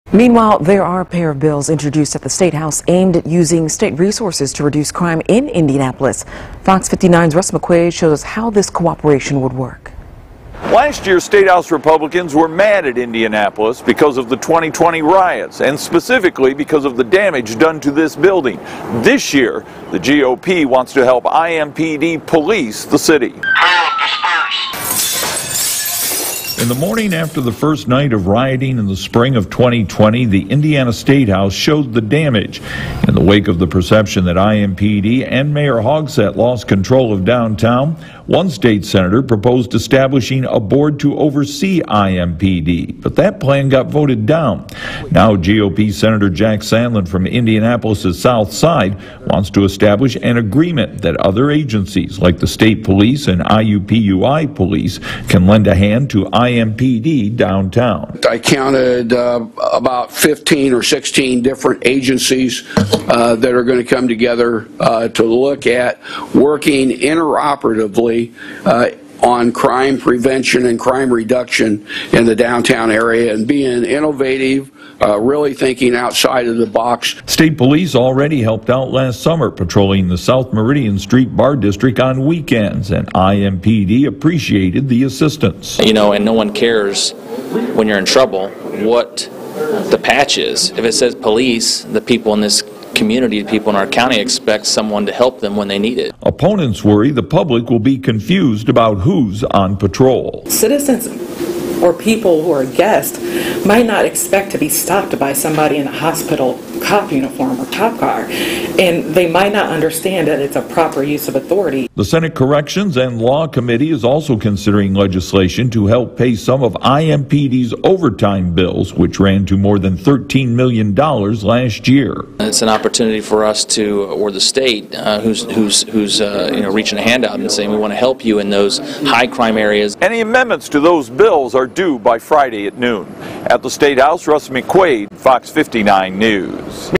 Legislation proposed to help IMPD pay for, patrol downtown
The spring riots of 2020 that left Monument Circle defaced with graffiti, scarred the doors of the Indiana Statehouse and saw the state’s capitol city choked with tear gas and ringing with gunfire so incensed Republicans in control of the state legislature that one of them proposed establishing a board to oversee the administration of IMPD.